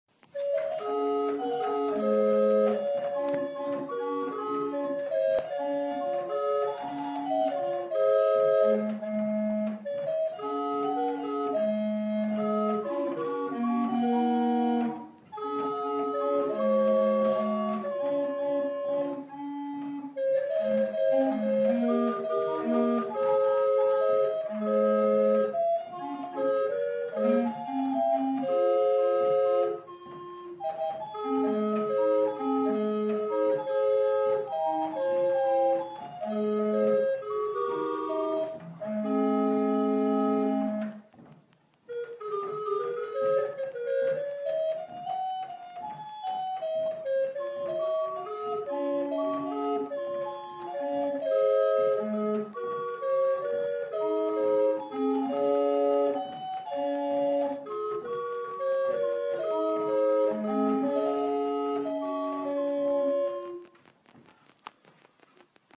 George Pyke barrel organ in Chippendale case ↘️
Barrel organ by George Pyke, Royal clockmaker and organ builder to Geo111 c1772. Housed in a mahogany case believed to be almost certainly from the workshop of Thomas Chippendale Snr.
Please check our website for full details of this instrument.
The only other known survivor of this size and complexity belonged to the Marquess of Anglesey and was sold in 1906 by Christie’s for 454 Guineas at the dispersal sale of the family seat Plas Newydd.
The whereabouts of this organ remained a mystery , but while studying images of the case work it was Identified in storage witin the bowels of the V&A London, England.